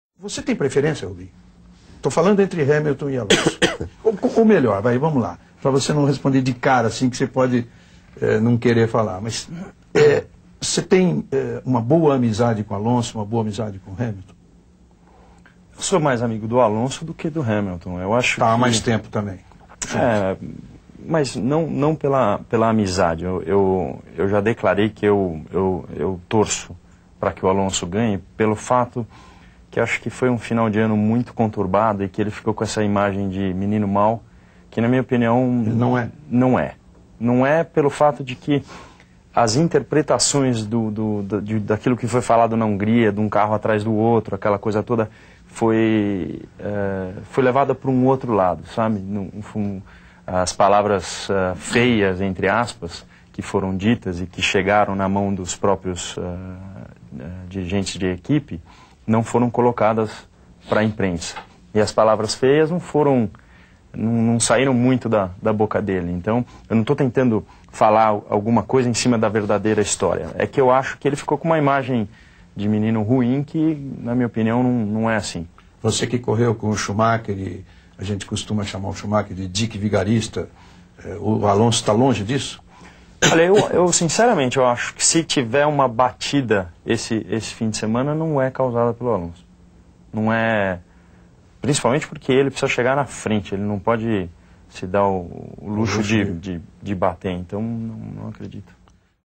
Declarações Rubinho sobre Alonso
Rubens Barrichello: `Eu torço pra que o Alonso ganhe no GP Brasil 2007`